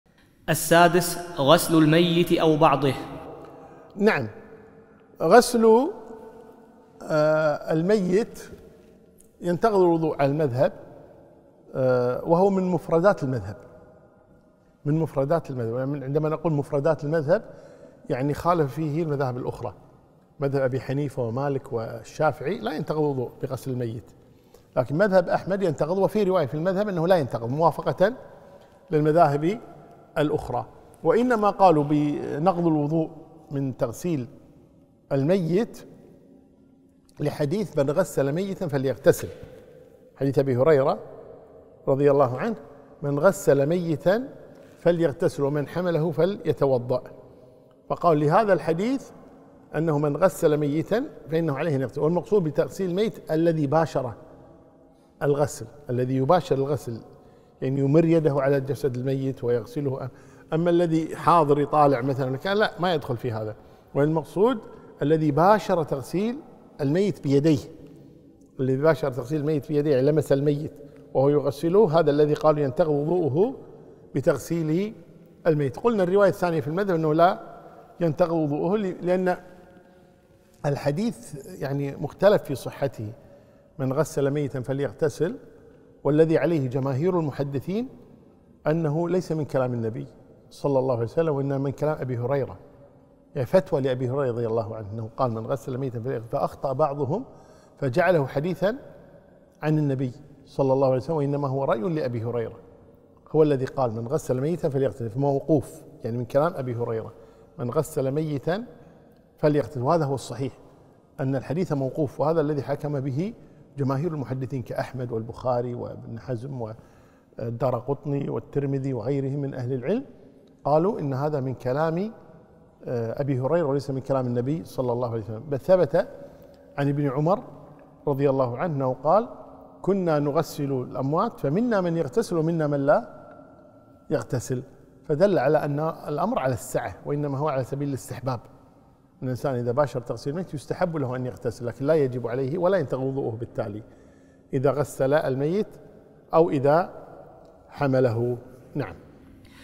229 - هل غسل الميت أو حمله ينقض الوضوء؟ - عثمان الخميس
5️⃣
المجلس الخامس

للاطلاع على جميع دروس شرح كتاب تلخيص مختصر المقنع